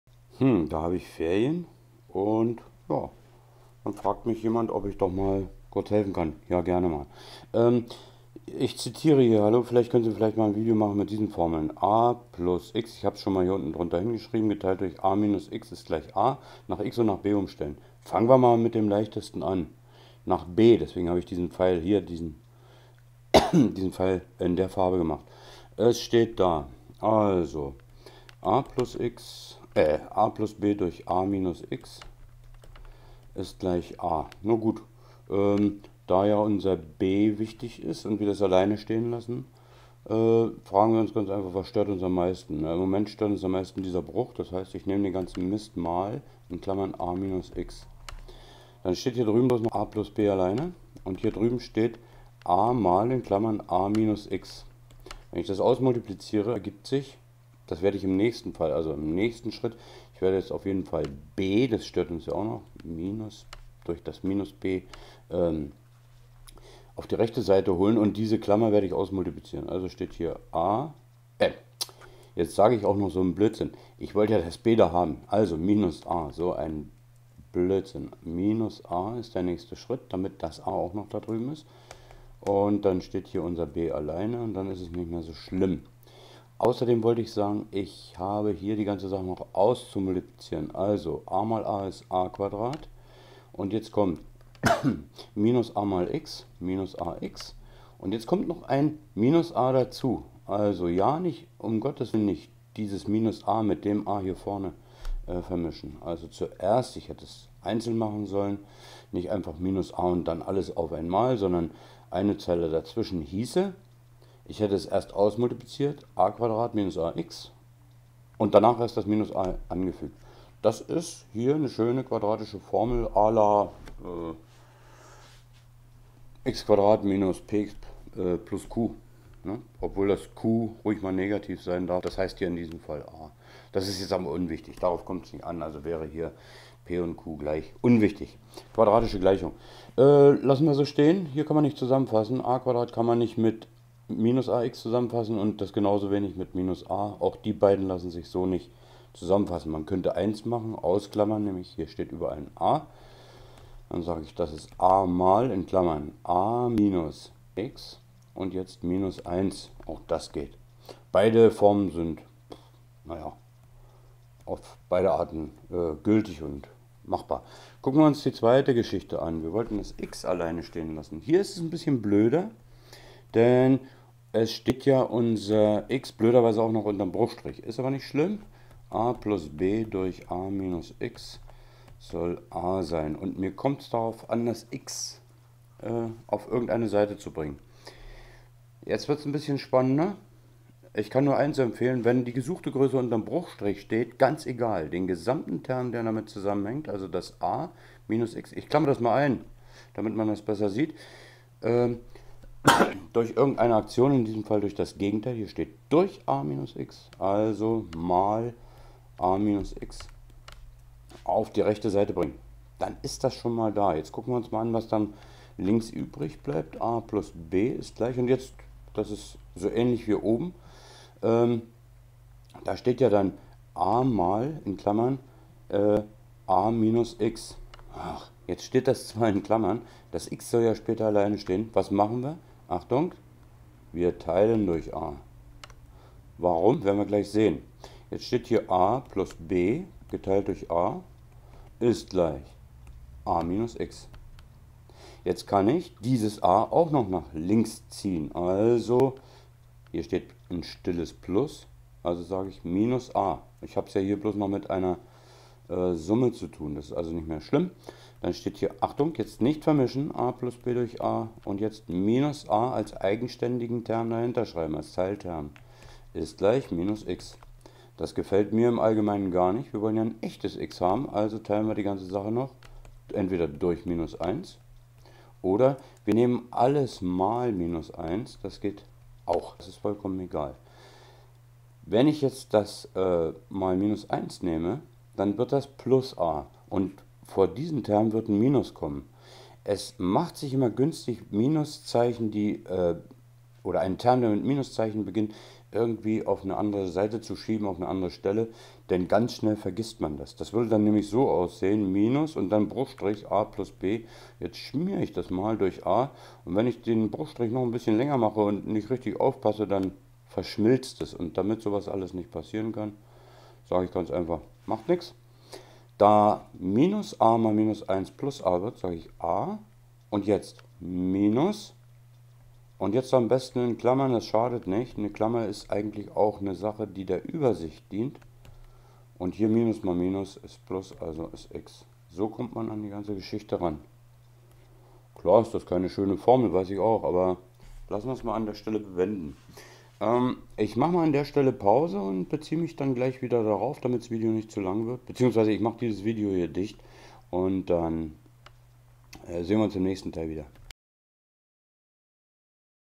Gleichungen (Formeln) (auf)lösen, wenn X ganz blöd steht (Bruchgleichungen) Teil 1
Bitte, helfen Sie uns Endlich wieder (Sapere aude!) Algebra.  Immer Schritt für Schritt, also einfach: An sich kein Problem, wenn man cool bleibt und lieber ein paar Zeilen mehr schreibt.

Nebenbei: Viele Leute sagen auch "X-Rechnen" oder "X-Rechnungen" statt "Gleichungen lösen".

07 2015_08_15 :zzzzz_intern